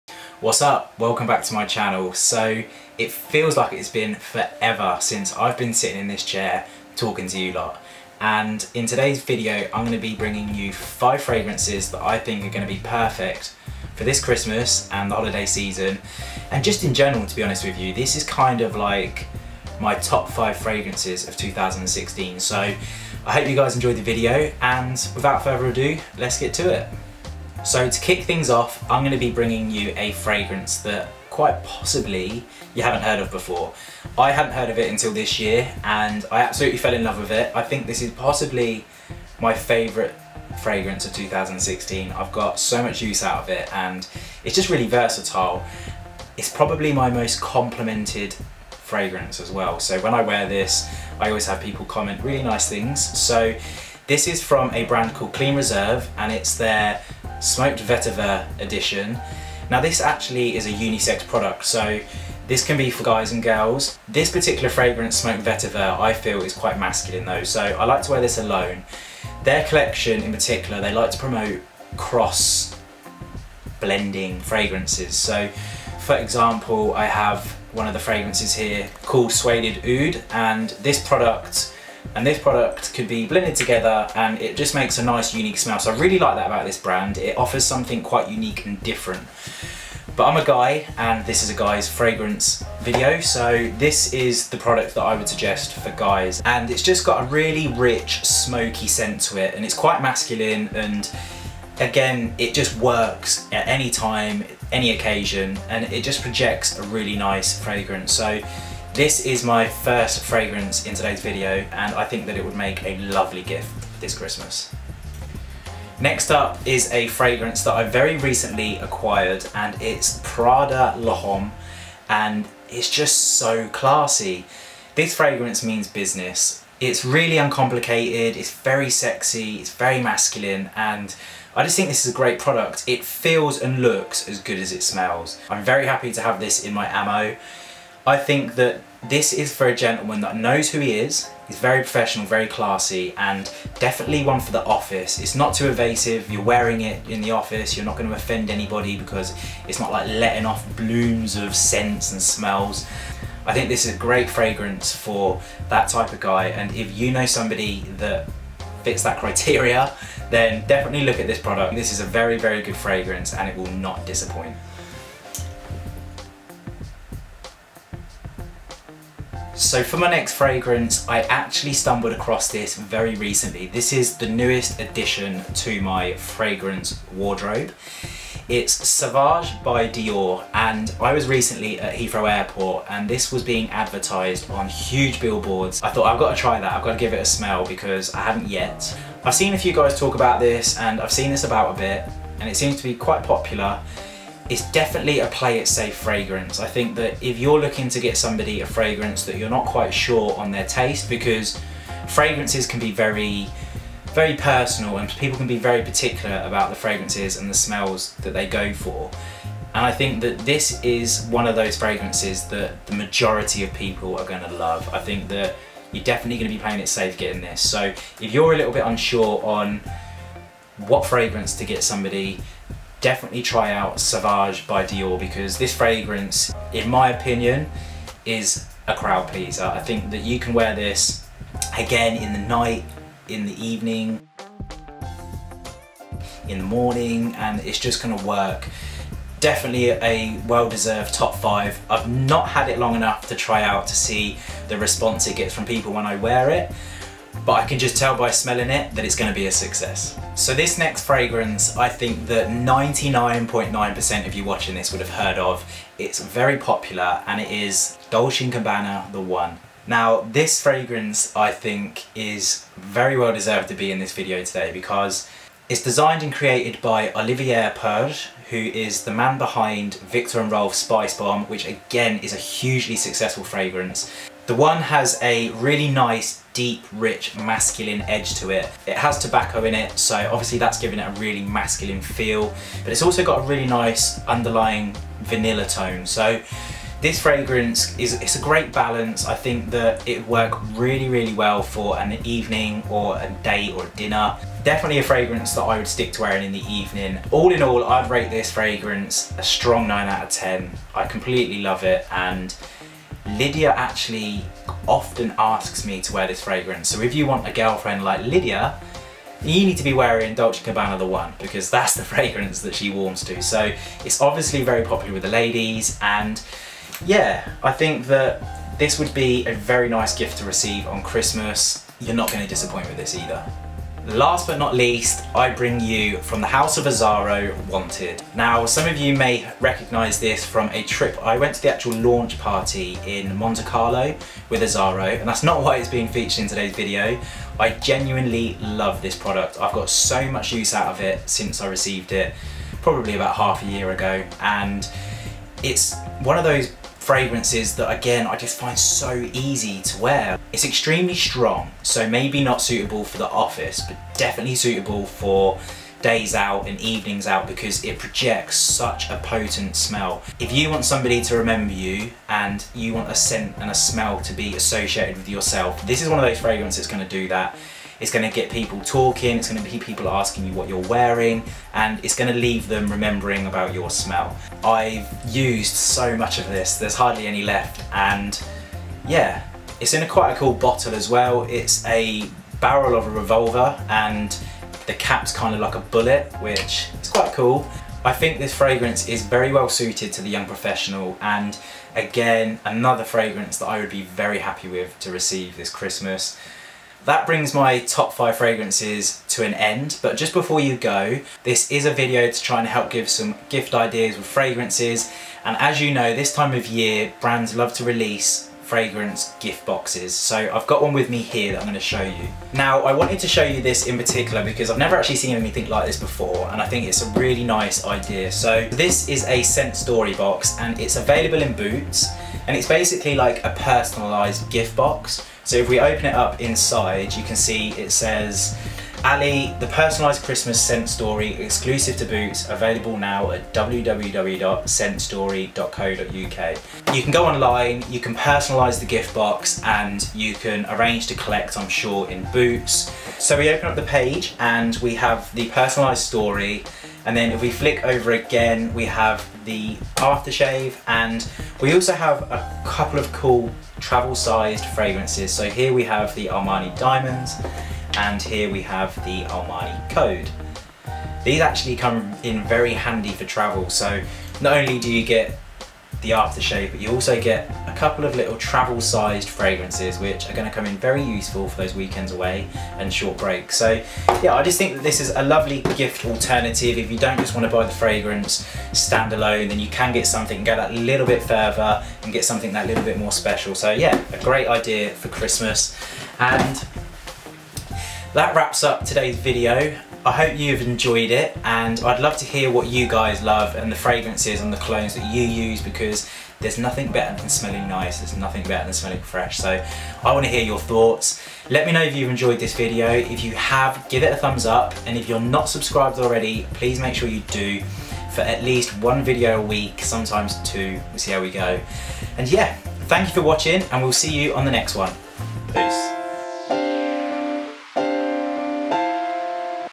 5 Fragrances for the man in your Life | Ali Gordon
With the Holidays on our door step I though what better time than now to give a little gift idea inspo & put together a few fragrances that have left a mark on me in 2016! I want to hear your thoughts & Fragrances you Love. - AG

Fragrances  :

Clean Reserve Smoked Vetiver -
Prada L'homme
Dior Sauvage
D&G The One
Azzaro Wanted

I own the Licensing to this track -Epidemic Sounds